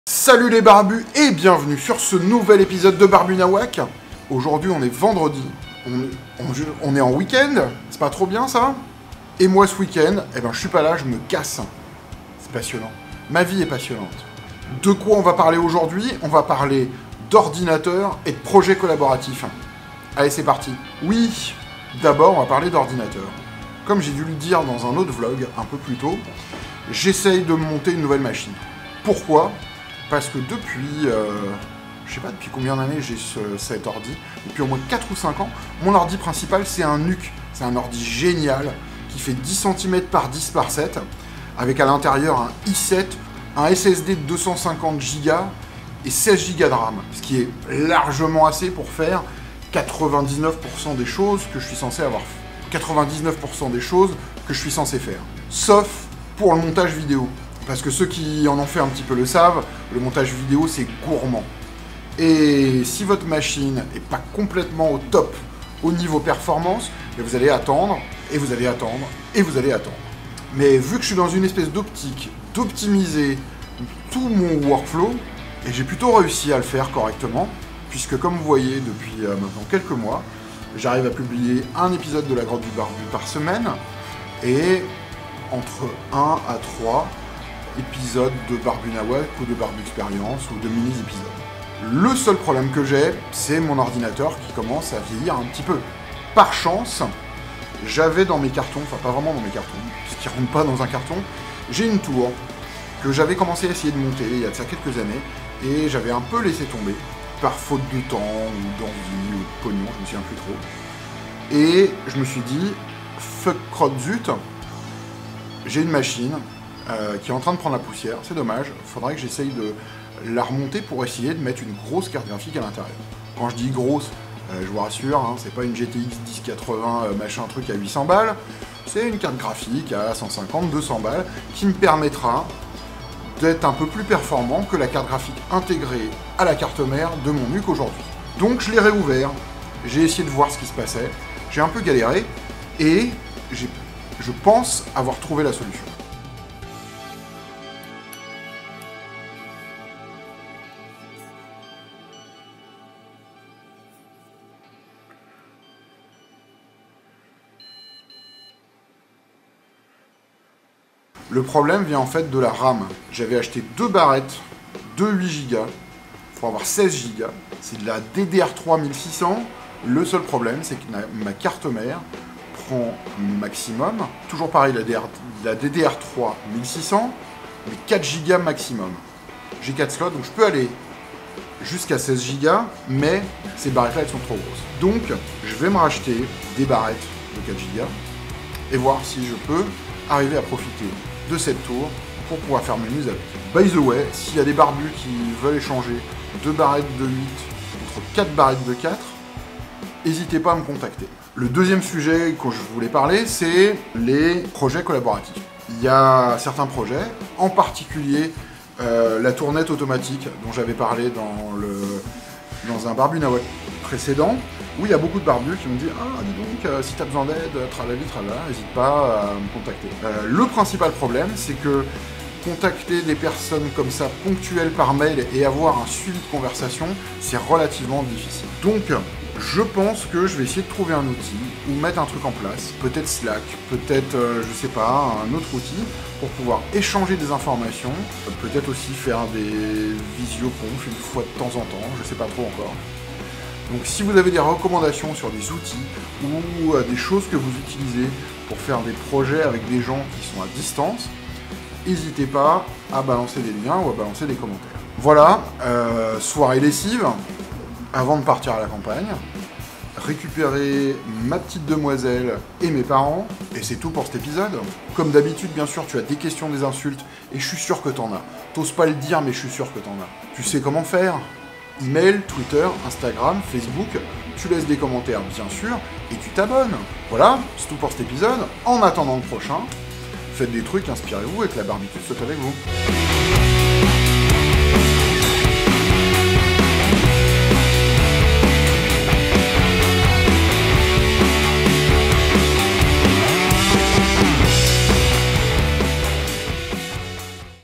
RAM pour ordi, projets collaboratifs, aller retour à la campagne... - barbuNawak
Et oui j'essaye de faire revivre une vieille tour, si vous avez 4x4 Go de RAM en DDR3 je suis  intéressé et j'ai aussi 2x8Go à refourguer :D